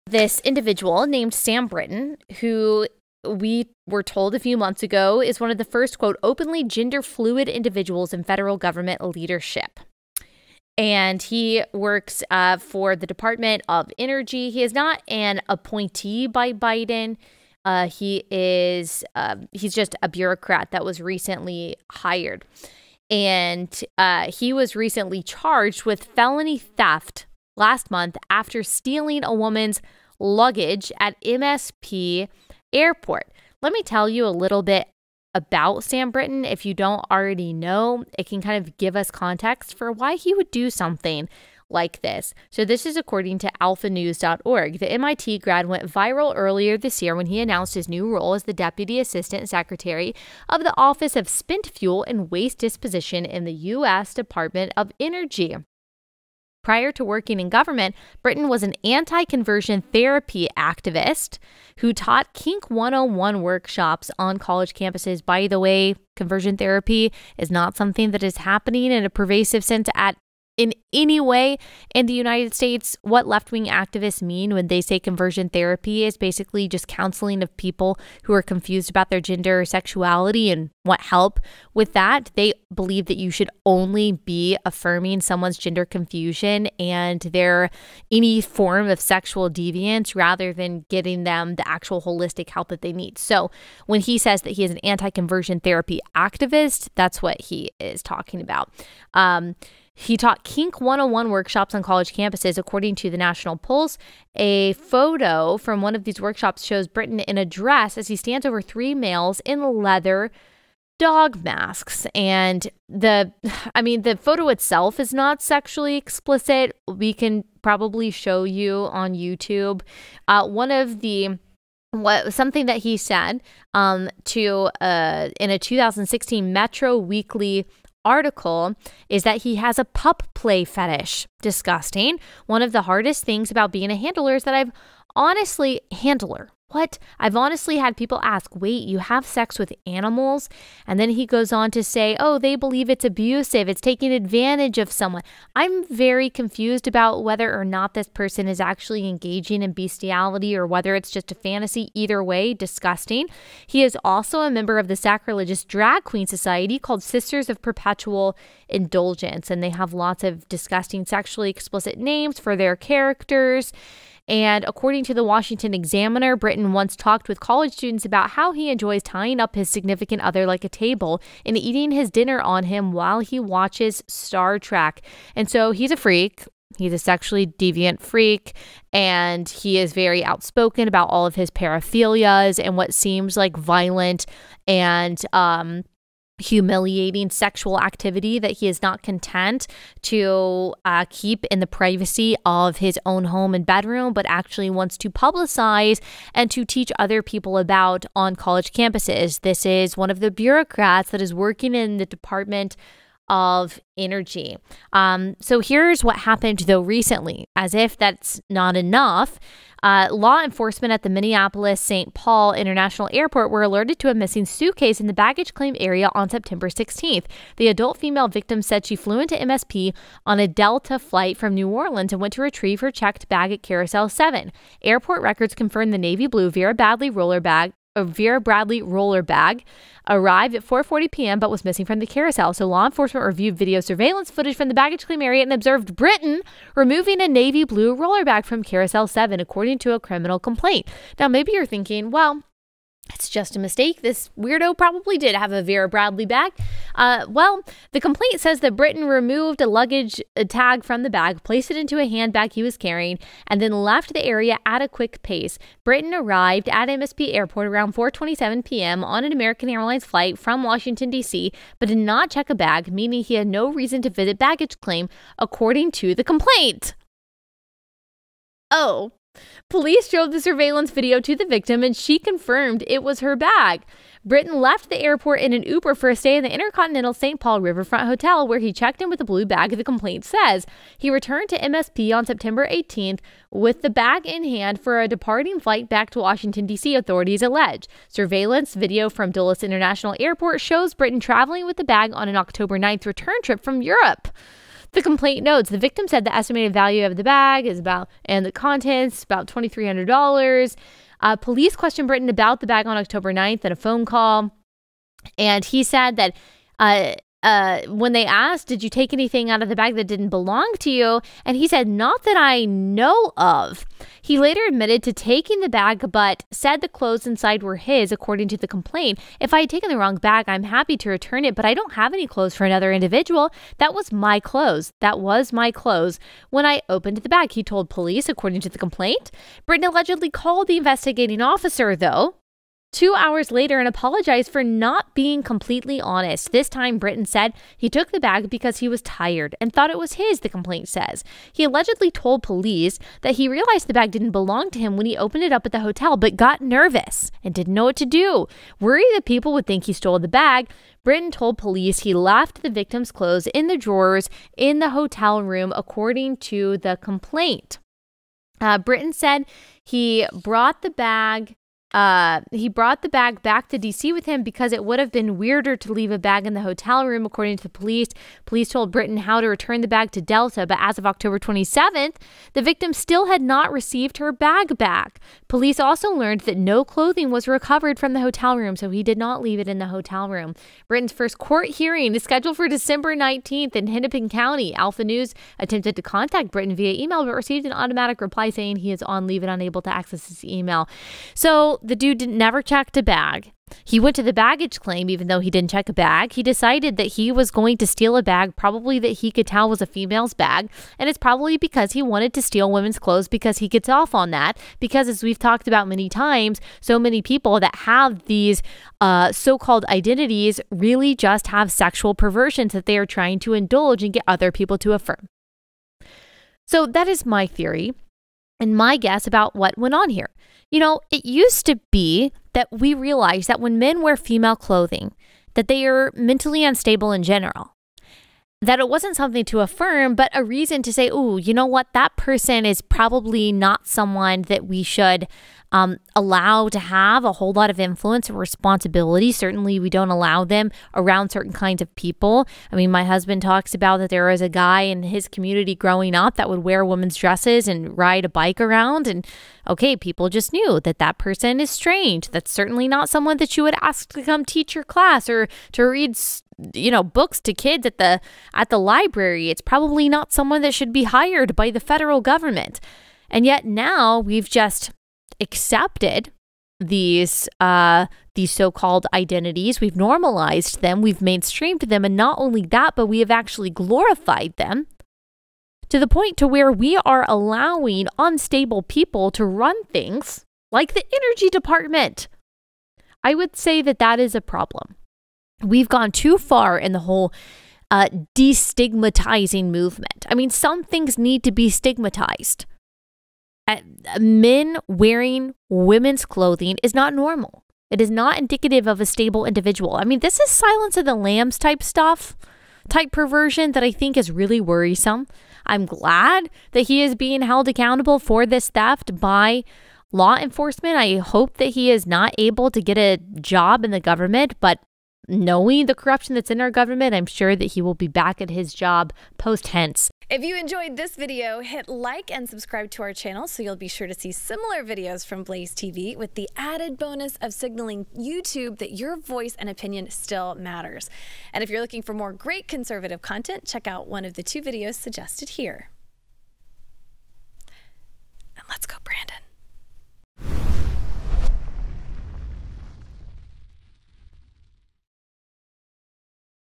Biden's Gender-Fluid Official CAUGHT Stealing Woman's Luggage
Biden's "gender-fluid" nuclear official Sam Brinton, was just charged with stealing a woman's luggage at the airport. @AllieBethStuckey says this is what happens when these identities are normalized and glorified.